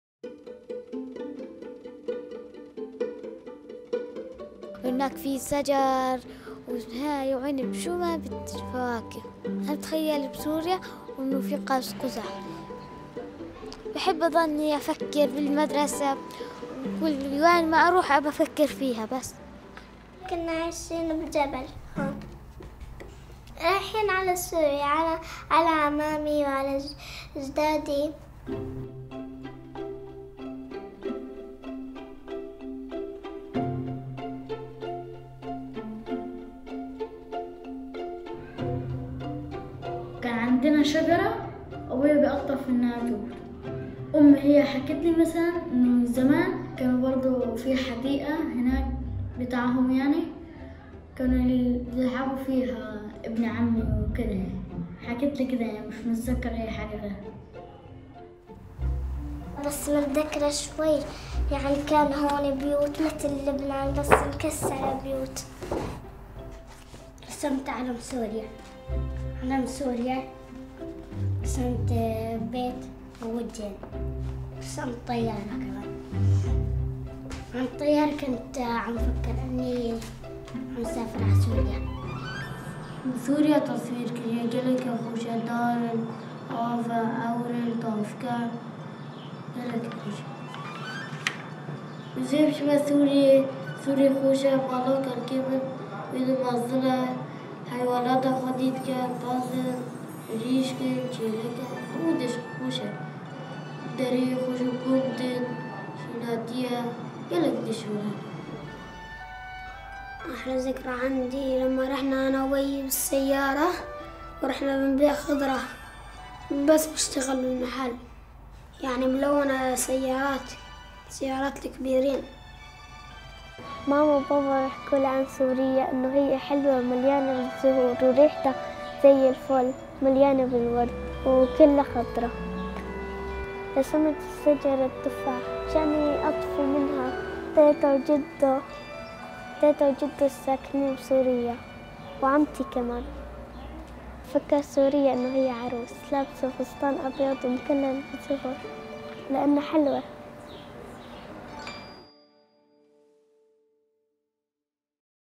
Imaginando Siria - Durante 11 años, estas niñas y niños solo han podido soñar con su país de origen
Toda una generación de niñas y niños sirios está creciendo como refugiados en los países vecinos que nunca han visto o no tienen ningún recuerdo de su país de origen.